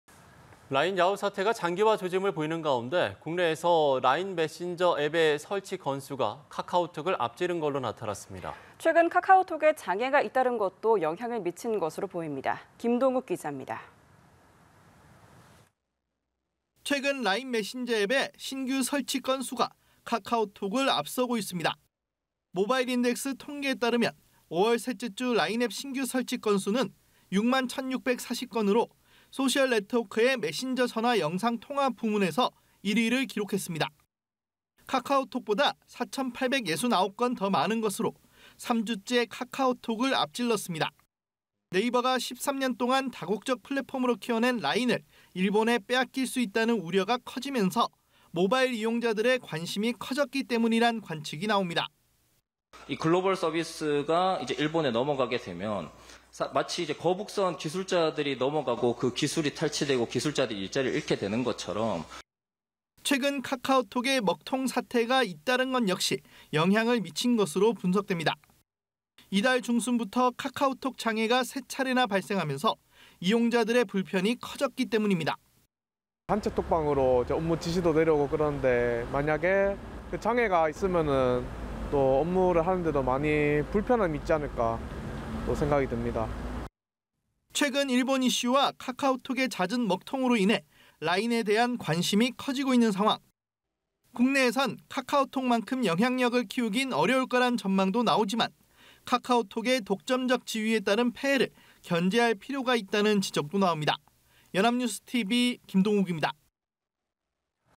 일본 매각 논란과 카톡 장애에…'라인 메신저' 설치 급증 / 연합뉴스TV (YonhapnewsTV)
일본 매각 논란과 카톡 장애에…'라인 메신저' 설치 급증

[앵커]

'라인야후 사태'가 장기화 조짐을 보이는 가운데 국내에서 라인 메신저 앱의 설치 건수가 카카오톡을 앞지른 것으로 나타났습니다.

최근 카카오톡의 장애가 잇따른 것도 영향을 미친 것으로 보입니다.

김동욱 기자입니다.

[기자]

최근 라인 메신저 앱의 신규 설치 건수가 카카오톡을 앞서고 있습니다.

모바일인덱스 통계에 따르면 5월 셋째 주 라인 앱 신규 설치 건수는 6만1,640건으로 소셜네트워크의 메신저·전화·영상통화 부문에서 1위를 기록했습니다.

카카오톡보다 4,869건 더 많은 것으로, 3주째 카카오톡을 앞질렀습니다.

네이버가 13년 동안 다국적 플랫폼으로 키워낸 라인을 일본에 빼앗길 수 있다는 우려가 커지면서 모바일 이용자들의 관심이 커졌기 때문이란 관측이 나옵니다.

[오세윤 / 네이버 노조 지회장] "이 글로벌 서비스가 일본에 넘어가게 되면 마치 거북선 기술자들이 넘어가고 기술이 탈취되고 기술자들이 일자리를 잃게 되는 것처럼…."

최근 카카오톡의 먹통 사태가 잇따른 것 역시 영향을 미친 것으로 분석됩니다.

이달 중순부터 카카오톡 장애가 3차례나 발생하면서 이용자들의 불편이 커졌기 때문입니다.

[제준영 / 부산 해운대구] "단체 카톡방으로 업무 지시도 내려오는데 장애가 있으면 업무하는데도 많은 불편함이 있지 않을까 생각합니다."

최근 일본 이슈와 카카오톡의 잦은 먹통으로 인해 라인에 대한 관심이 커지고 있는 상황.

국내에선 카카오톡 만큼 영향력을 키우긴 어려울 거란 전망도 나오지만, 카카오톡의 독점적 지위에 따른 폐해를 견제할 필요가 있다는 지적도 나옵니다.

[영상취재기자 홍종원]